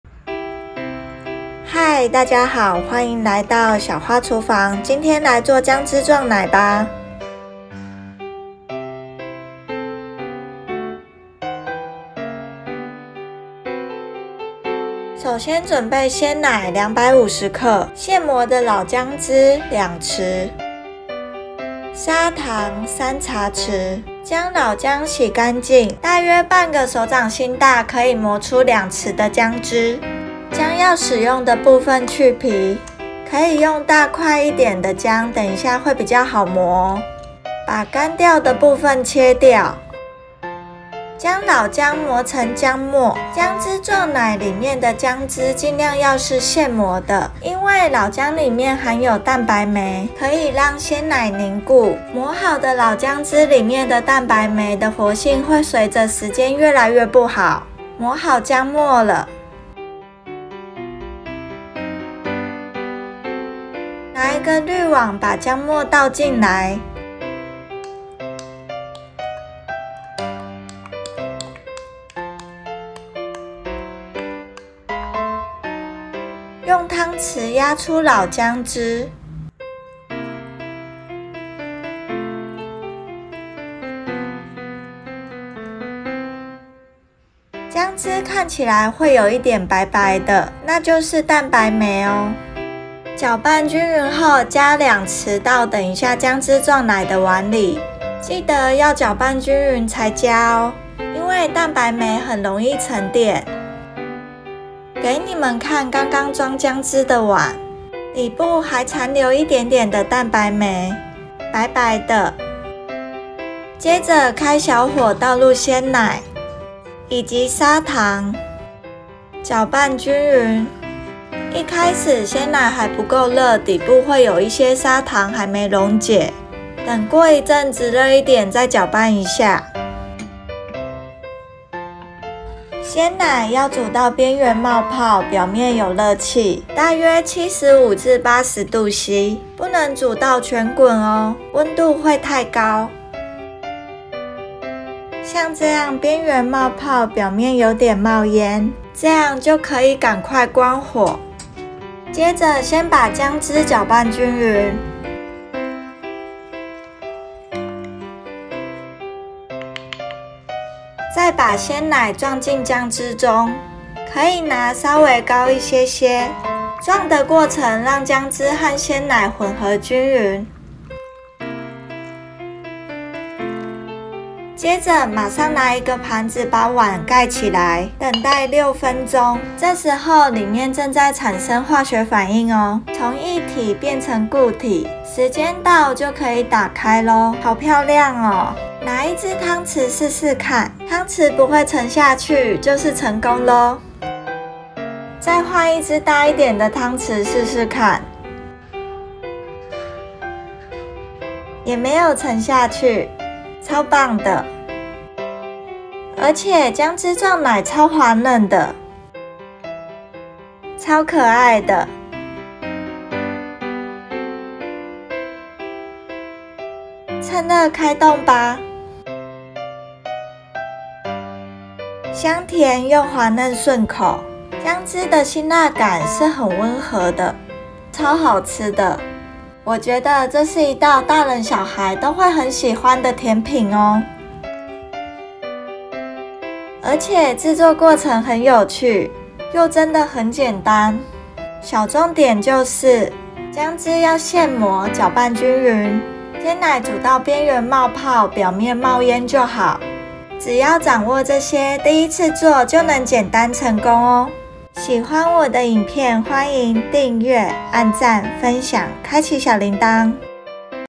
薑汁撞奶｜超簡單一次就成功！薑汁加鮮奶就變成奶凍了！好神奇好好吃～香甜滑嫩
薑汁撞奶 (Ginger Milk Pudding) 是港式經典的甜品之一，做法非常簡單又神奇，當薑汁撞上鮮奶，居然就變成奶凍了！而且整體香甜滑嫩，超好吃的😋 所有成功的小秘訣都在這了，跟著一起做，100%成功～

材料：
鮮奶 250g
現磨的老薑汁 2匙 (約12g)
砂糖 3茶匙 (約9g)